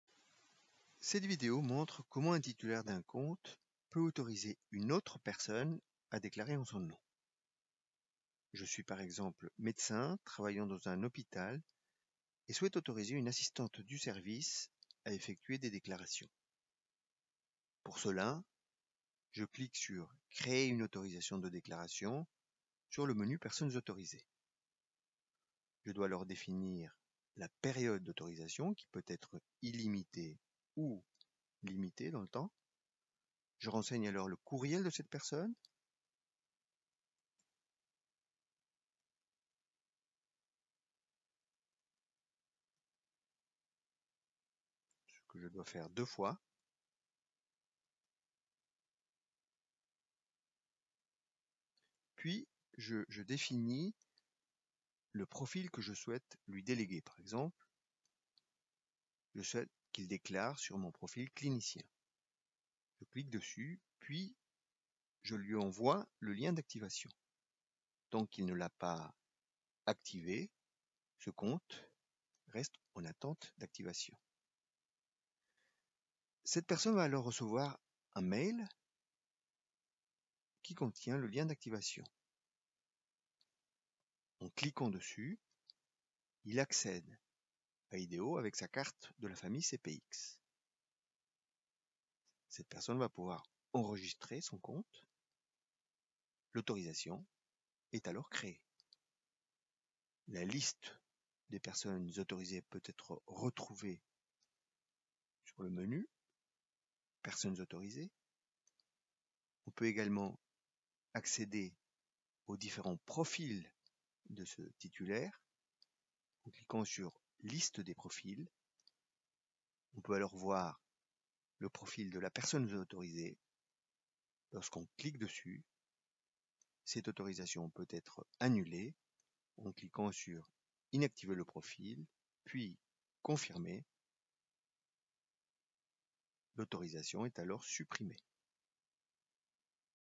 [e-DO] - Tuberculose : autoriser autre personne à déclarer
Cette vidéo montre comment faire lorsqu’un déclarant souhaite autoriser une autre personne à déclarer sur son profil sur l'application e-do.